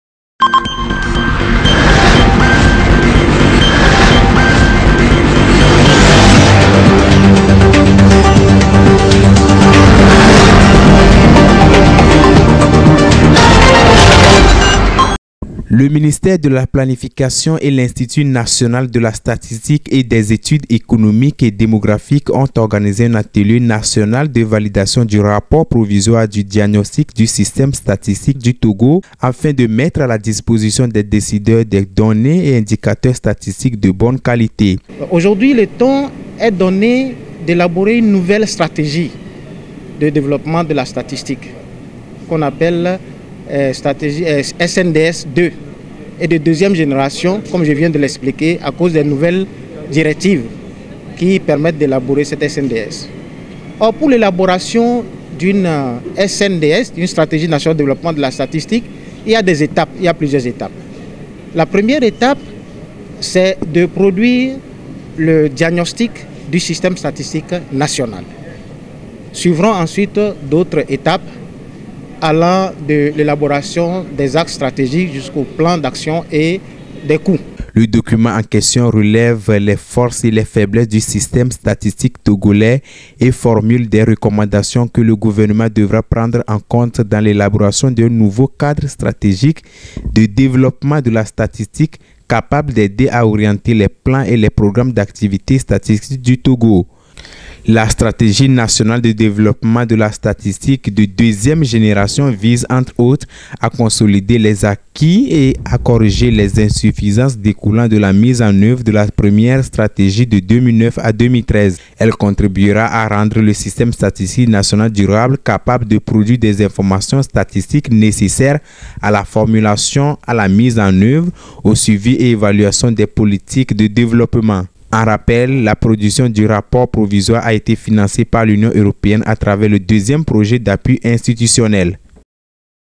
Vers une nouvelle Stratégie nationale de développement statistique
Les acteurs impliqués dans l’analyse des données statistiques du Togo passent au peigne fin le contenu du document afin de mettre à la disposition des décideurs des données et indicateurs statistiques de bonnes qualités.